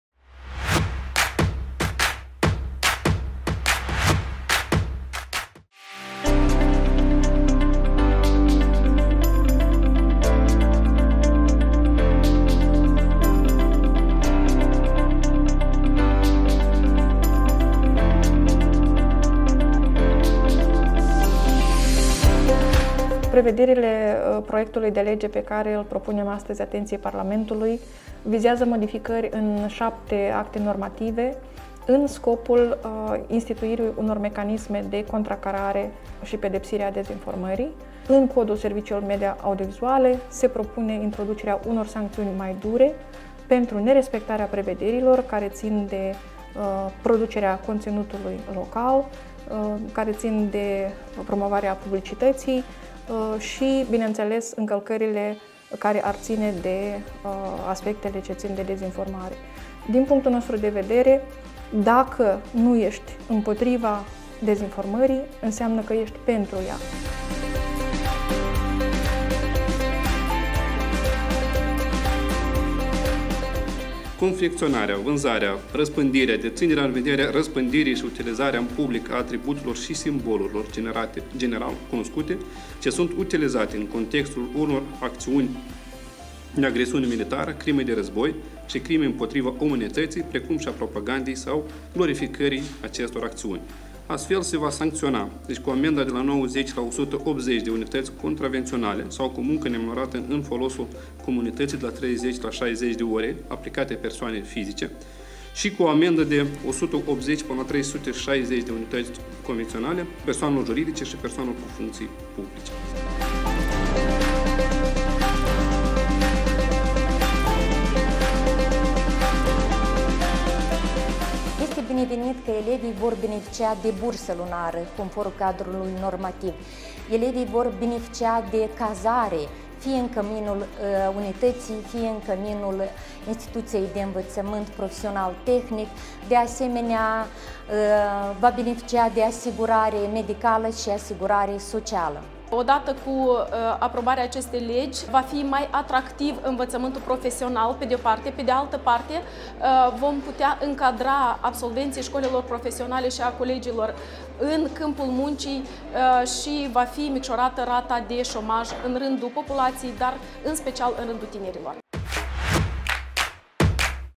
Sinteza sedinței parlamentului din 07.04.2022
Votat în Parlament:
✅ Legea Anti-Falsuri: Statul va avea mai multe instrumente să combată și să pedepsească propaganda și dezinformarea care pune în pericol securitatea statului.
✅ Se interzic simbolurile și atributele care promovează acțiuni de agresiune militară, crime de război și crime împotriva umanității. Persoanele care le răspândesc, inclusiv în mediul online, vor fi amendate.
✅ Cea de-a treia zi de sâmbătă a lunii aprilie, declarată „Ziua comemorării victimelor foametei organizate”.
✅ Învățământul dual: Elevii vor beneficia de bursă lunară, de cazare, asigurare medicală și asigurare socială. Absolvenții școlilor profesionale și a colegiilor vor putea fi încadrați în câmpul muncii. Astfel, va fi micșorată rata de șomaj în rândul tinerilor.
✅ Creditare nebancară responsabilă: Se introduc mecanisme de protecție a consumatorilor care iau credite de la organizații de microfinanțare.
✅ Un pas important pentru securitatea energetică! Furnizorii de gaze naturale vor fi obligați să aibă stocuri de gaze naturale pentru cel puțin 2 luni de iarnă.
✅ Modificări importante la Codul contravențional: Amenzi mai dure pentru incendierea pășunilor și pădurilor, pentru desfășurarea construcțiilor neautorizate.
✅ Moldova va împrumuta încă 30 milioane euro pentru a izola termic clădirile publice de importanță socială.